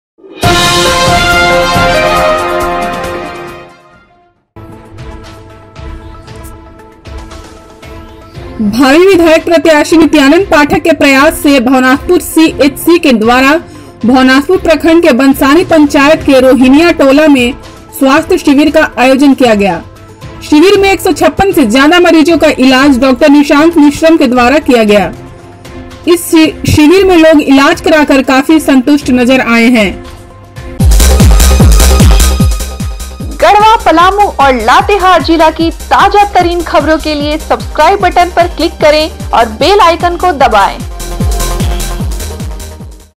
भवनाथपुर |गढ़वा | भावी प्रत्याशी नित्यानंद पाठक के प्रयास से स्वास्थ्य शिविर का आयोज
भावी विधायक प्रत्याशी नित्यानंद पाठक के प्रयास से भवनाथपुर CHC के द्वारा, भवनाथपुर प्रखंड के बनसानी पंचायत के रोहिनिया टोला में स्वास्थ्य शिविर का आयोजन किया गया। शिविर में  156 से ज्यादा मरीजों का इलाज DR. निशंक निश्रम के द्वारा किया गया। इस शिविर में लोग इलाज कराकर काफी संतुष्ट नज़र आये हैंLatest News
Breaking News
Current News
garhwa news
palamu news
latehar news
Talash TV